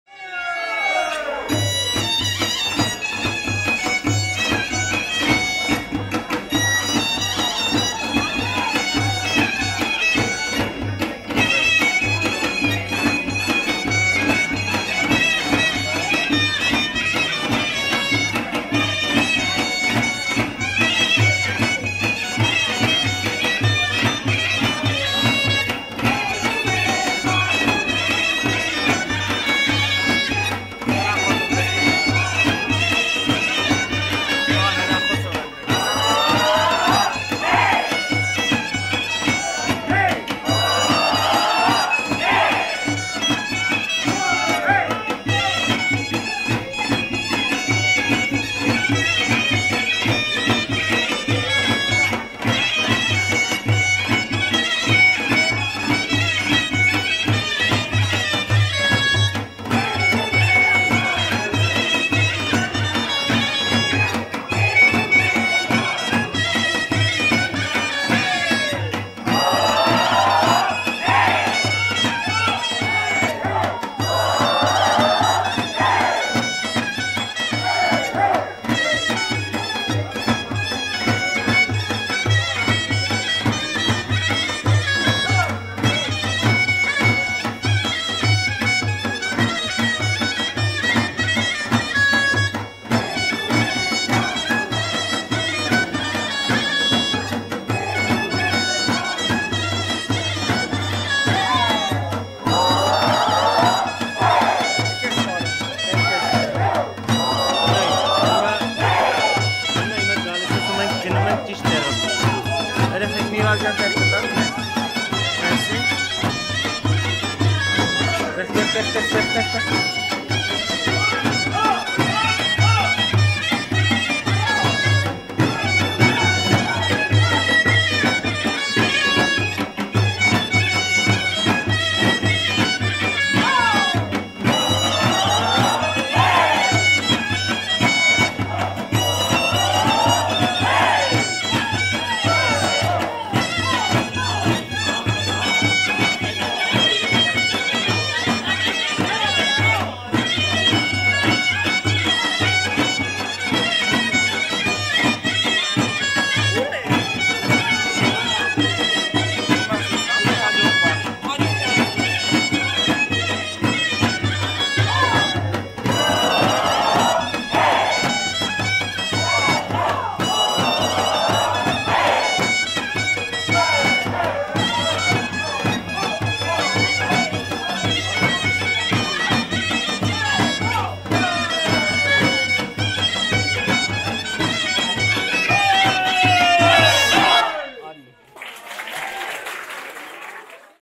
Էջմիածին - ՄԵՐ ՊԱՐԵՐԸ ԵՎ ՄԵՆՔ (ՀՈՒՆՎԱՐ - 2024)
«Մեր պարերը և մենք» հայ ավանդական պարերի բաց դաս-միջոցառում:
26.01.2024
Նարեկացի արվեստի միություն
Հանդիպումից այլ հատվածներ․

Պարային միջոցառումների և համերգների մասին կարող եք տեղեկանալ հետևելով